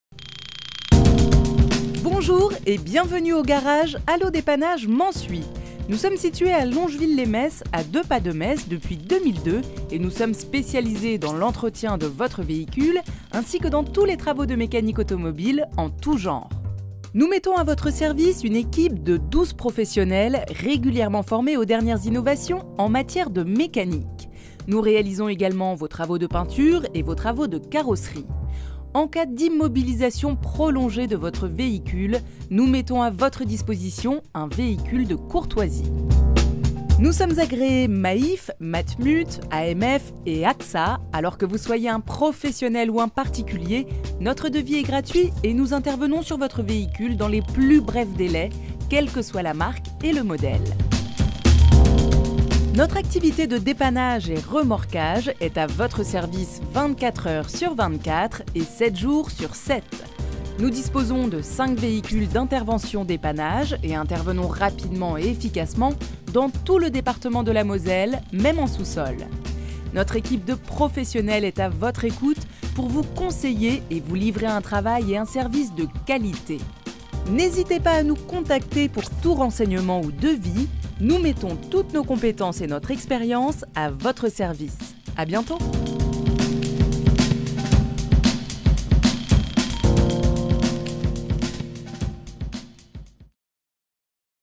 Allo dépannage Mansuy : Garage dépannage auto moto Metz 57
Bienvenue au garage Allo Dépannage Mansuy. Situés à Longeville les Metz à deux pas de Metz depuis 2002 et nous sommes spécialisés dans l'entretien de votre véhicule .Nous mettons à votre service une équipe de 12 professionnels, régulièrement formés aux dernières innovations en matière de mécanique. Nous réalisons également vos travaux de peinture et  carrosserie. En cas d'immobilisation prolongée de votre véhicule, nous mettons à votre disposition un véhicule de courtoisie.Nous sommes agréés Maif, Matmut, AMF et Axa alors que vous soyez un professionnel ou un particulier, notre devis est gratuit et nous intervenons sur votre véhicule dans les plus brefs délais quelle que soit la marque et le modèle.Notre activité de dépannage et remorquage est à votre service 24h/24 et 7 jours sur 7. Nous disposons de 5 véhicules d'intervention-dépannage et intervenons rapidement et efficacement dans tout le département de la Moselle, même en sous-sol.